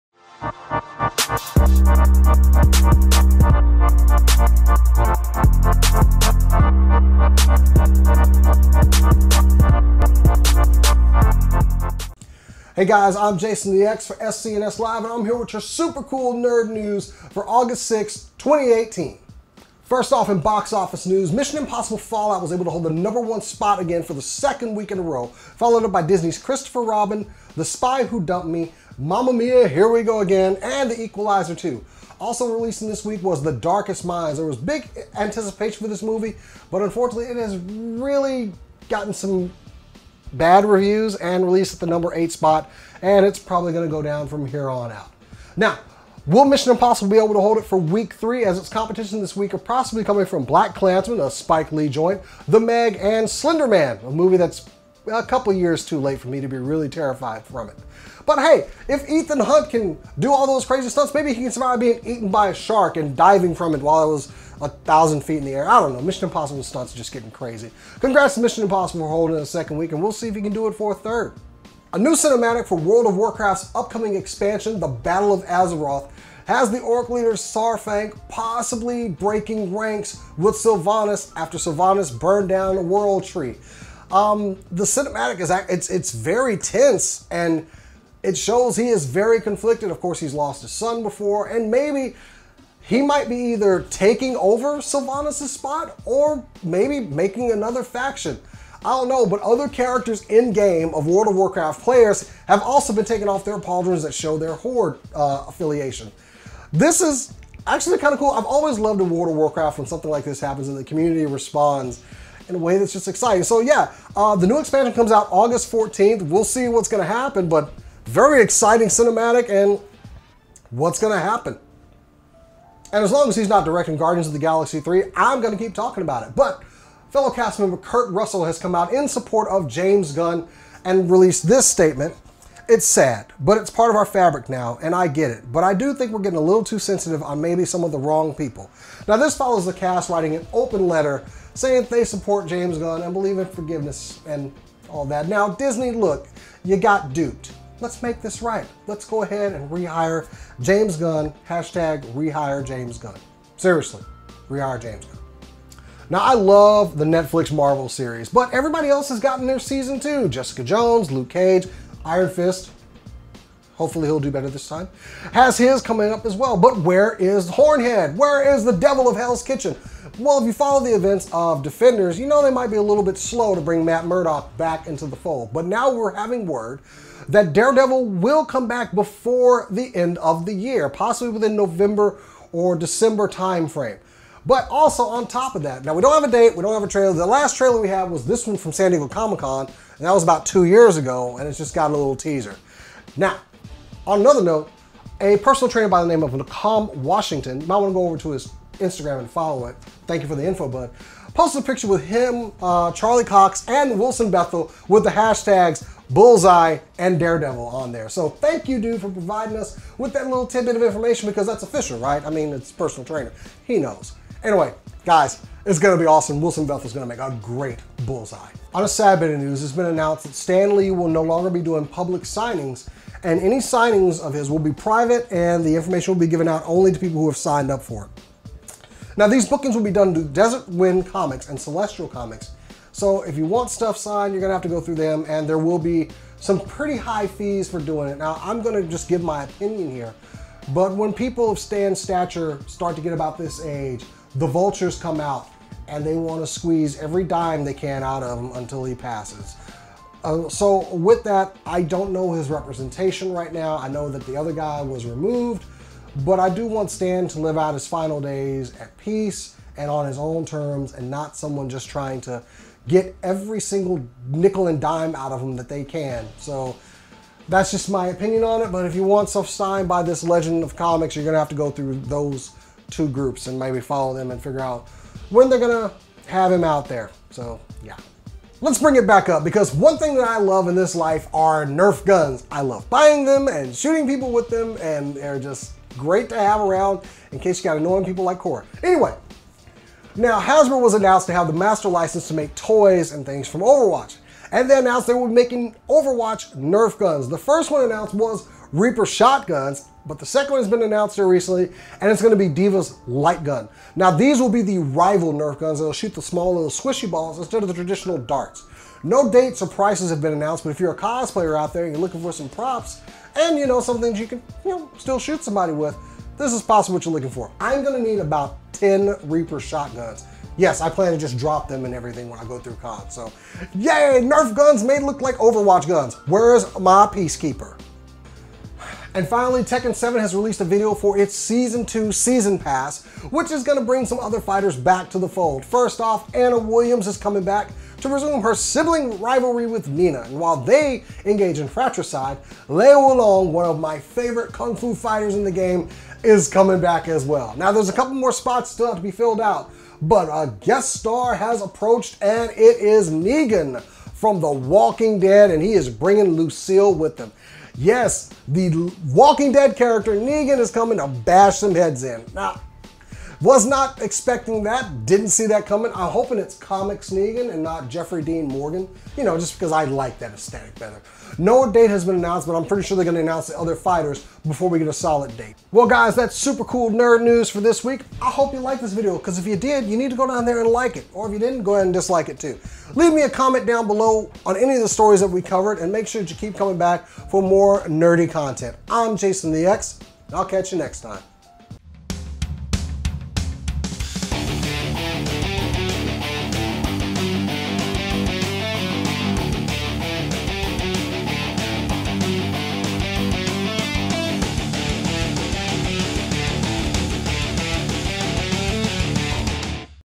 Super Cool Nerd News August 6th 2018
Welcome to the August 6th edition of Super Cool Nerd News. We take a look at the box office this week. A new cinematic for the upcoming expansion World of Warcraft :Battle of Azeroth was released and shows some breaks in the Horde ranks. Daredevil Season 3 news and when we can expect it. Stan Lee has been privatized and we'll tell you the details. Nerf is making guns from Overwatch. And lastly a new Tekken 7 season 2 season pass video showing us three fighters coming to the game.

for money off your 1st crate.